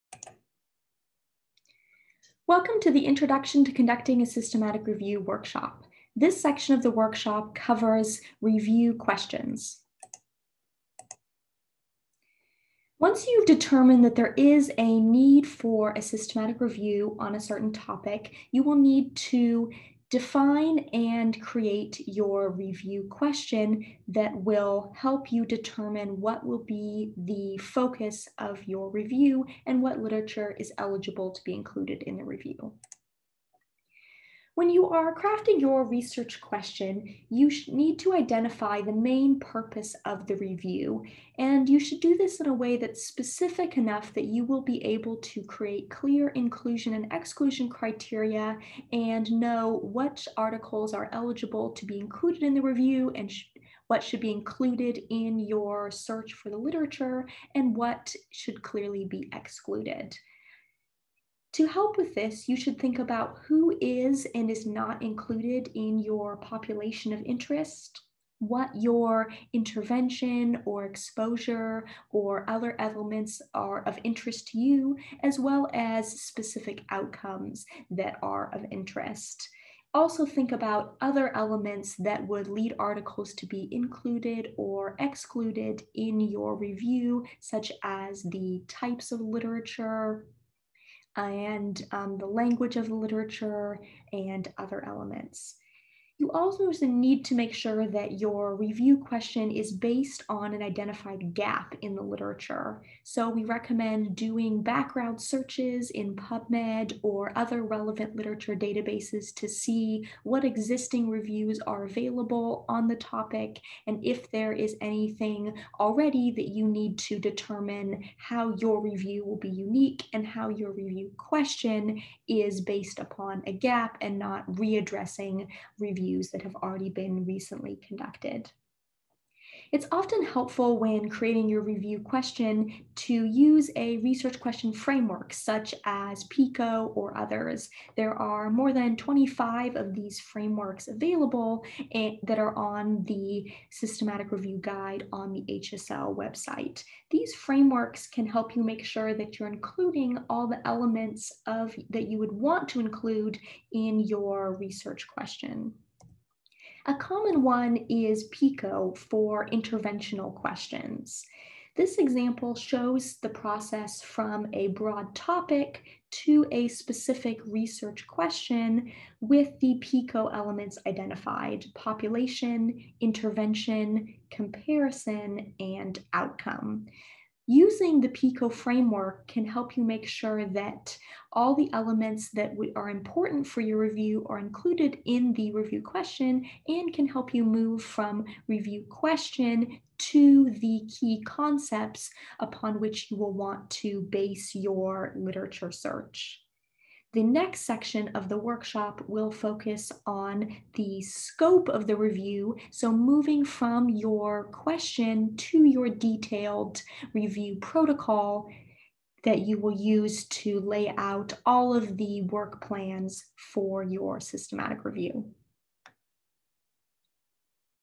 Introduction to Systematic Reviews workshop - part 2
The Introduction to Conducting a Systematic Review workshop will cover recommended standards, methods, and tools for completing a systematized, scoping, or systematic review at UNC. This workshop is recommended for those who have not yet conducted such a review, but are planning to do so within the next year. This is part two of the six part workshop.